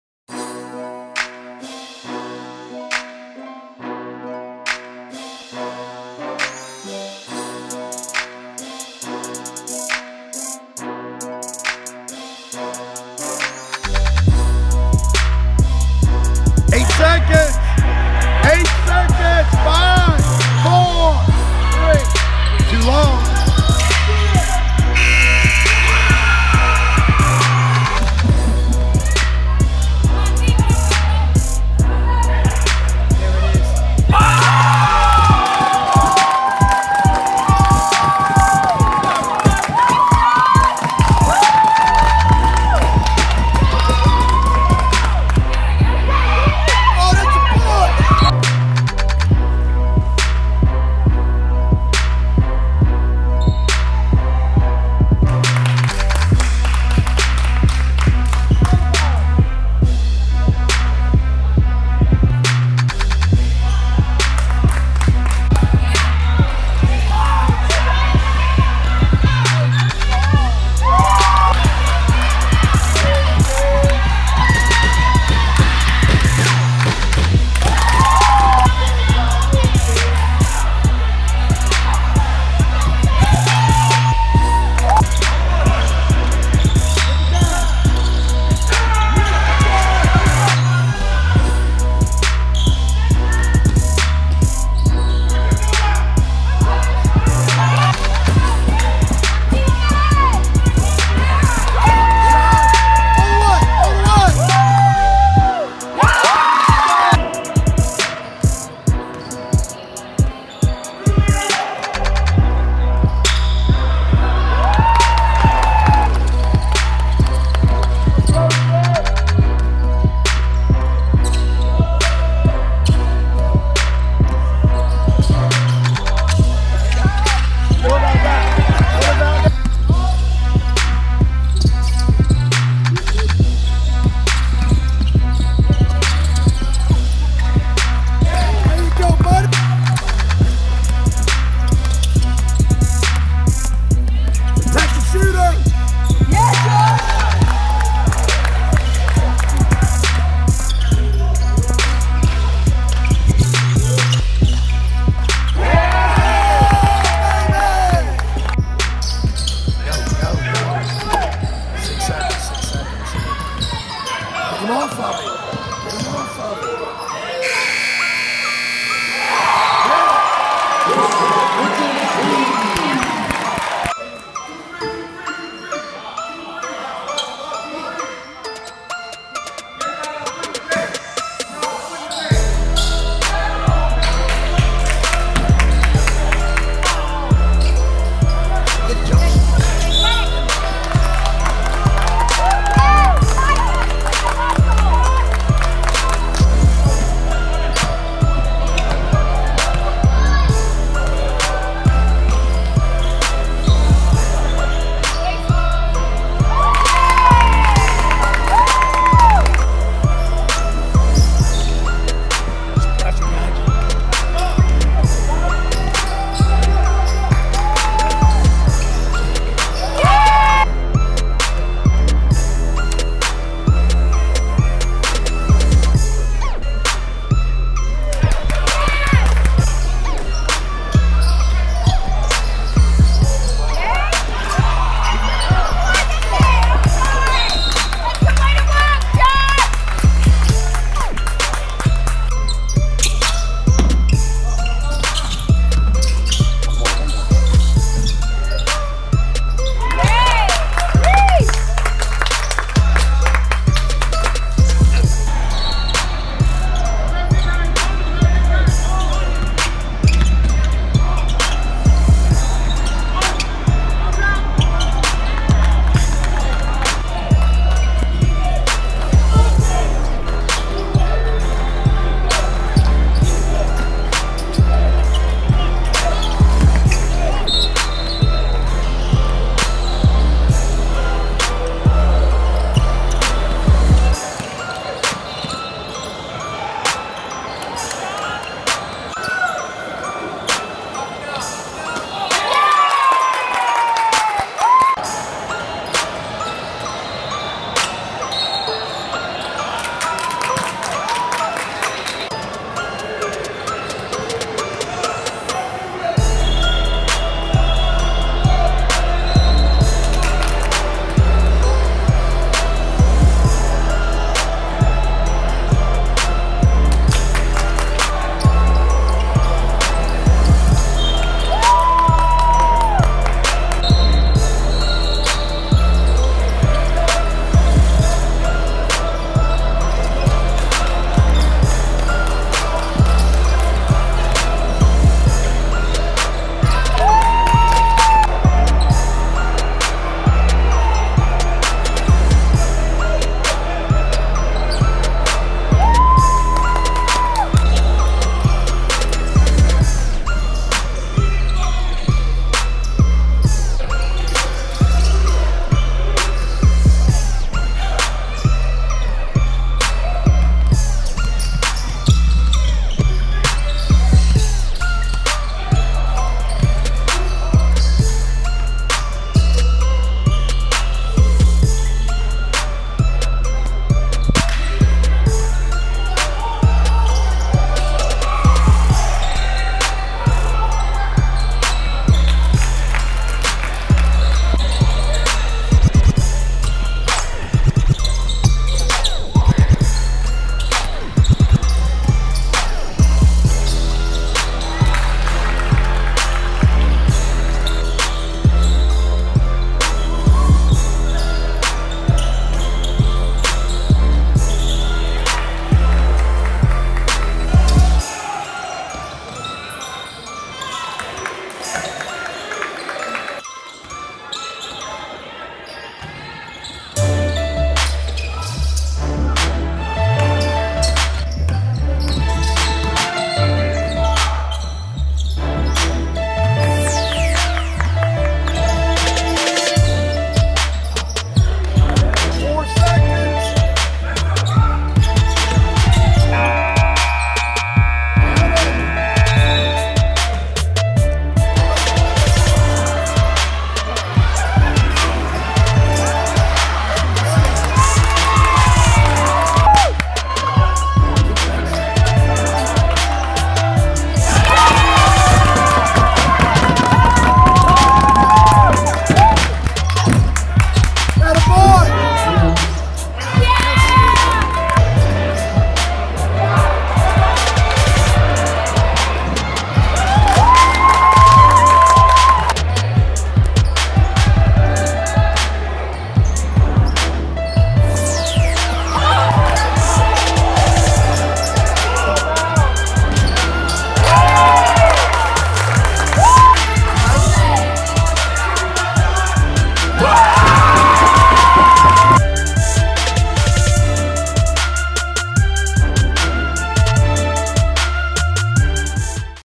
Josh Mims Basketball Highlights 2017-18
Josh Mims - Melbourne High School Varsity Point Guard, West Melbourne Select AAU Team, England National U15 Team

Video Created By: Courtney Mims